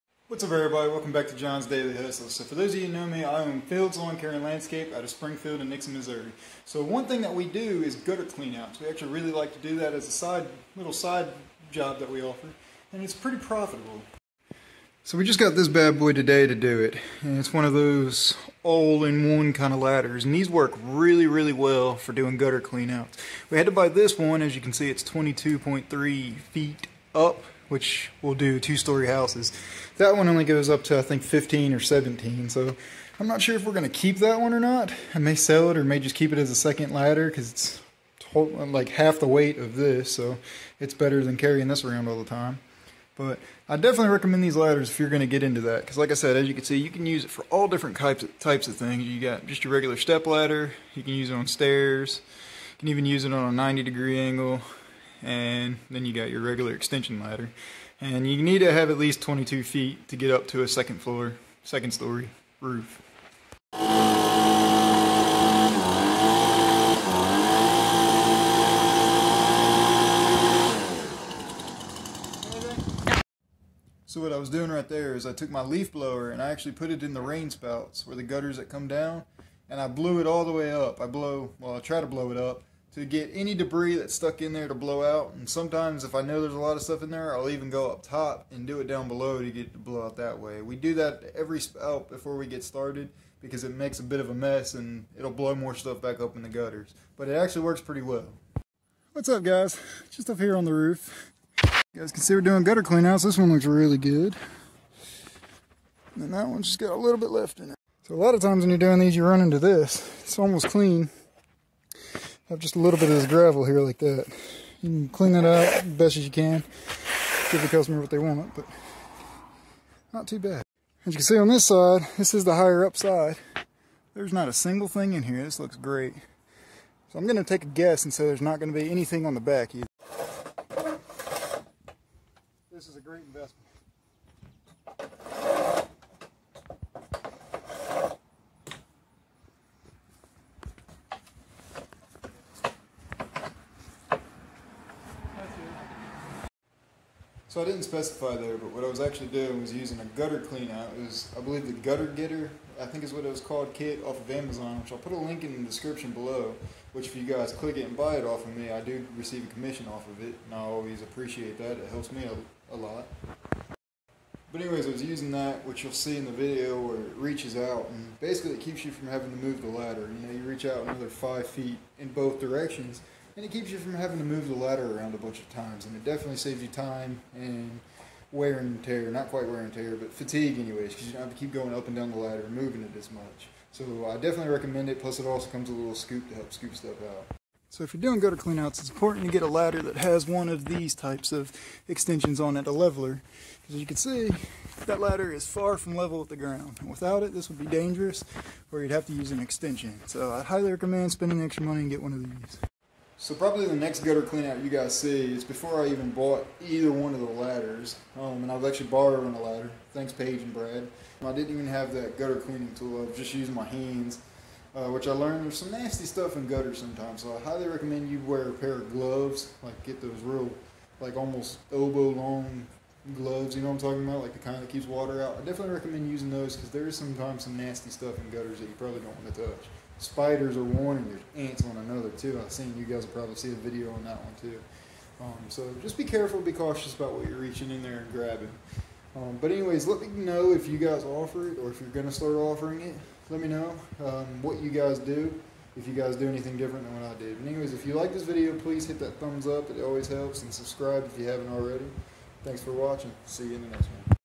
Lawn Care Vlog - Gutter Cleaning
Fields Lawn Care And Landscape offers Gutter cleaning in Springfield and Nixa Missouri. I will show you how I do it and show my new ladder I purchased

Gutter Getter (cleaning tools)
Gutter Cleaning Device